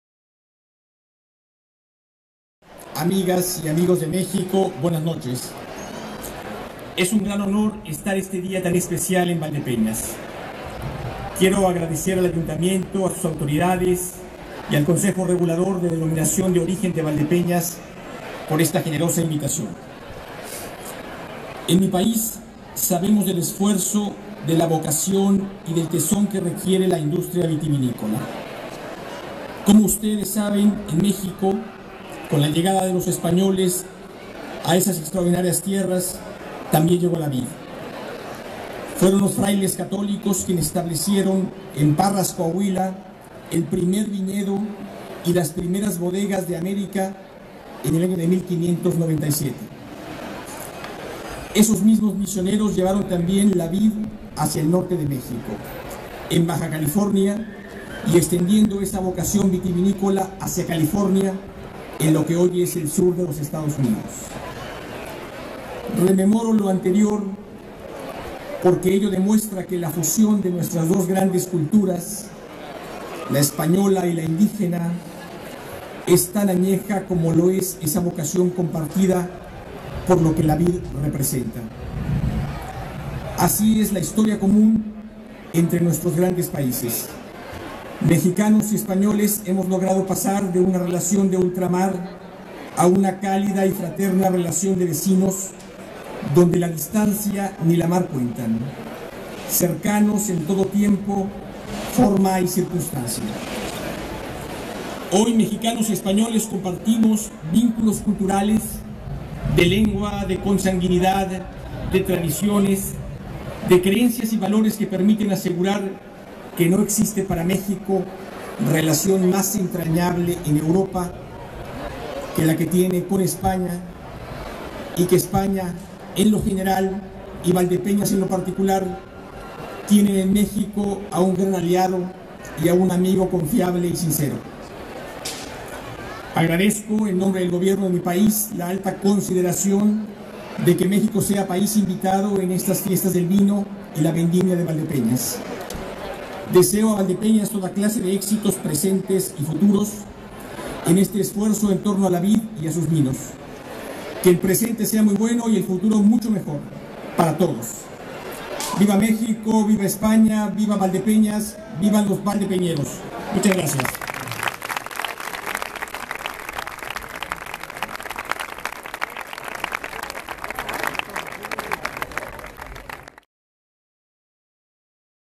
Discurso de Ernesto Sosa Gallegos en las LIX Fiestas del Vino en Valdepeñas
Discurso del representante de la Embajada de México en España, Ernesto Sosa Gallegos, durante el acto institucional de las LIX Fiestas del Vino en Valdepeñas, Ciudad Real, España.